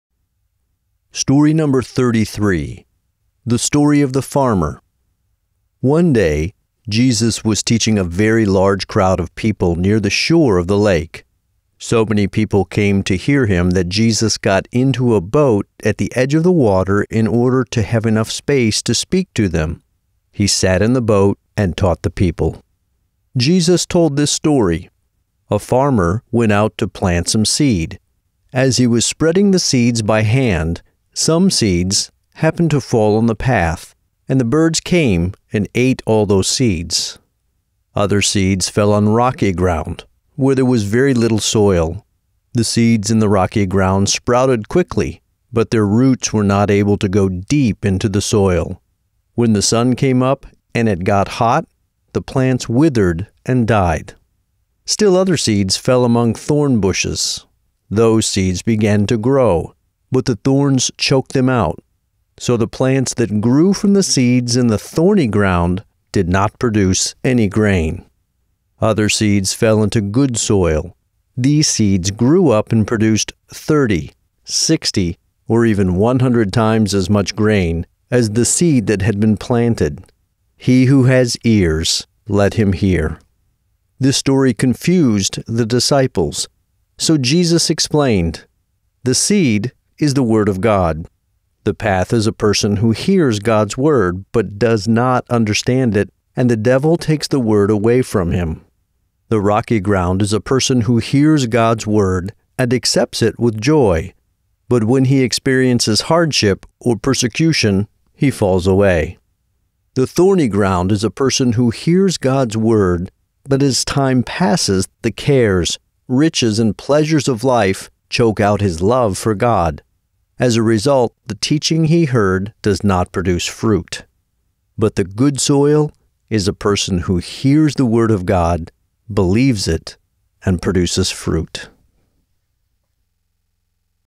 33. The Story of the Farmer - Open Bible Stories (v4)
Open Bible Stories by unfoldingWord
A Bible story from Matthew 13:1-8, 18-23; Mark 4:1-8, 13-20; Luke 8:4-15

Publisher: unfoldingWord
Producer/Editor: Brad Harrington
Assistant Editor: Ben Jore
3D Effects/Editor: Travis Blake